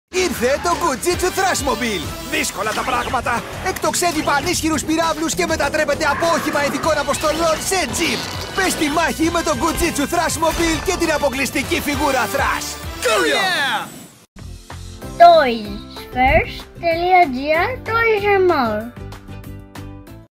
Goo Jit Zu Όχημα Thrash Mobile, www toysfirst gr
Ξεκινήστε τη δράση με τον Thrash και το φοβερό Thrash Mobile Όχημα!
Αυτό το Goo Jit Zu όχημα σε σχήμα καρχαρία μετατρέπεται από τζιπ σε όχημα ειδικών αποστολών με μια κίνηση!
Πιέστε προς τα κάτω για να τεντώσετε τους τροχούς και να κινηθείτε στη "Λειτουργία ταχύτητας" ή τραβήξτε προς τα πάνω το πλαίσιο και εξαπολύστε επίθεση στη "Λειτουργία κρούσης" και εκτοξεύστε πυραύλους από τα σαγόνια του καρχαρία!
Μαζί με το Thrash Mobile περιλαμβάνεται και μια αποκλειστική φιγούρα Race Suit Thrash για να τον τοποθετήσετε στη θέση του οδηγού.  Εξακολουθεί να είναι υπέρ-ελαστικός όπως πάντα, ο Thrash μπορεί να τεντωθεί μέχρι και 3 φορές και στη συνέχεια να επιστρέφει στο αρχικό του σχήμα και μέγεθος.